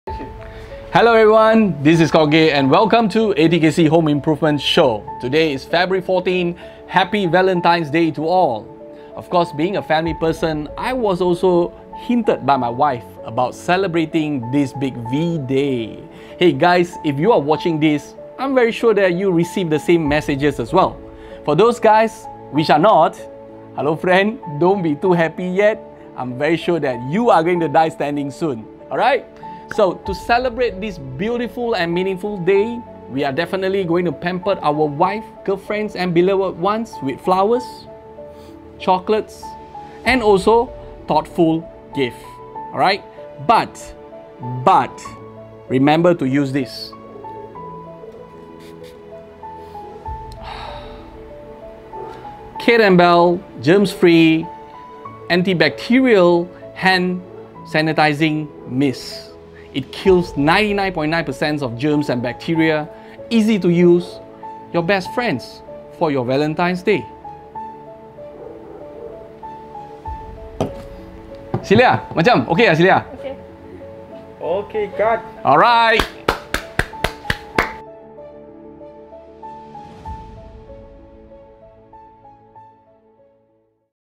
Happy Valentines Day with Kath+Belle Hand Sanitizing Mist
For those who are celebrating Valentines this year, stay safe & hygiene with Kate+Belle Hand Sanitizing Mist! 100% organic ingredients, anti-bacterial and ready to kill 99.9% germs & bacteria - the best gift for your loved ones!

- Malaysia's Largest Home Improvement Online Store.